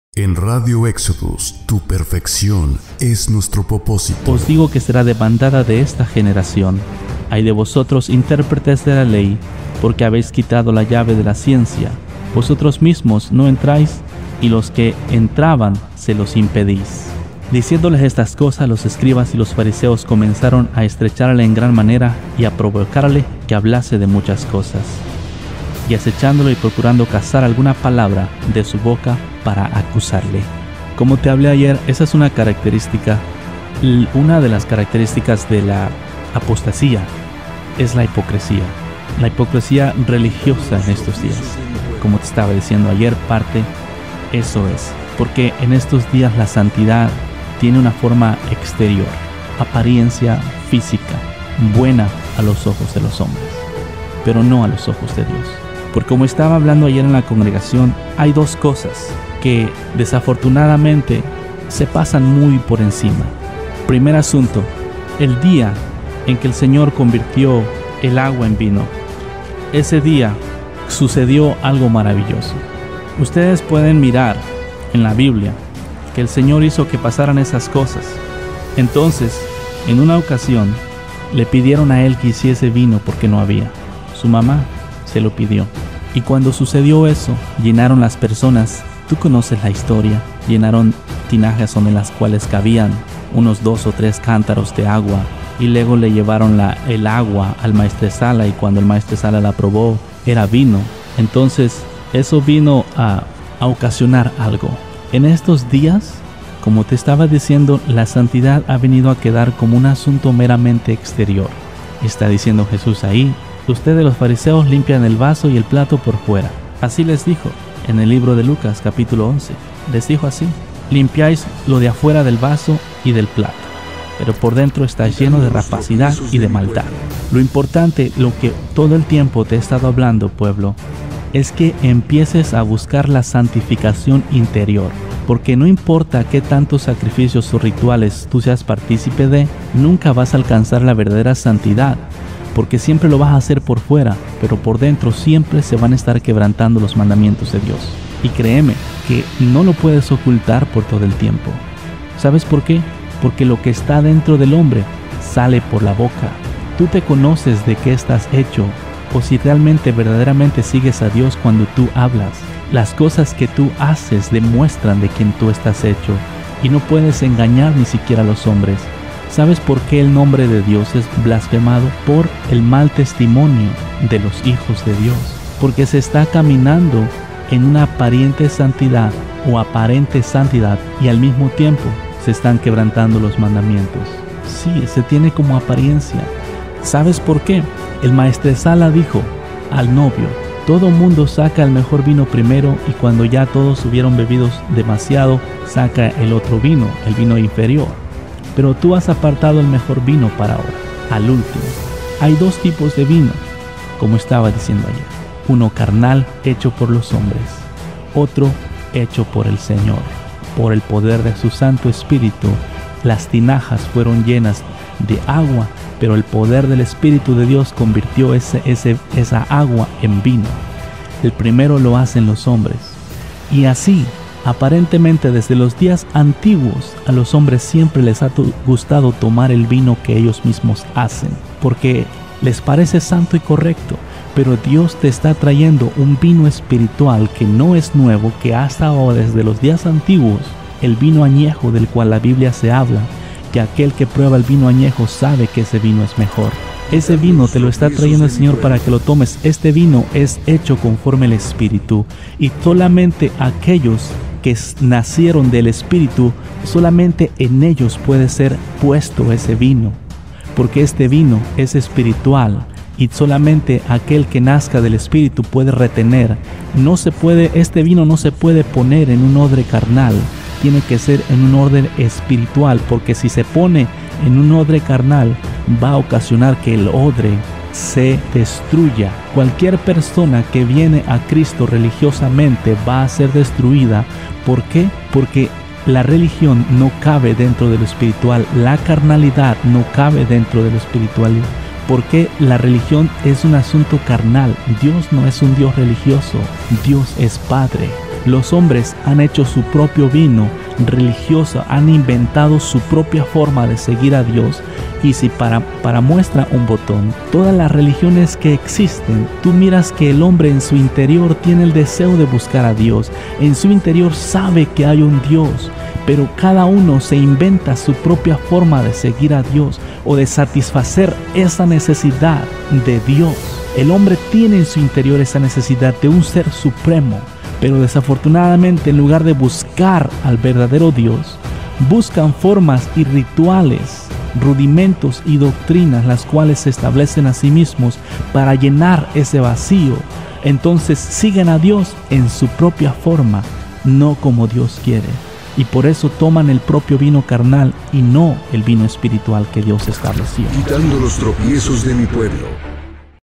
HAY DE VOSOTROS INTÉRPRETES DE LA LEY
Mateo 5:48
“Sed pues, vosotros perfectos, como vuestro Padre que está en los cielos es perfecto”
Hebreos 10:14
“Porque con una sola ofrenda hizo perfectos para siempre a los santificados. ”

¡Ser perfecto no es una opción, es un mandamiento!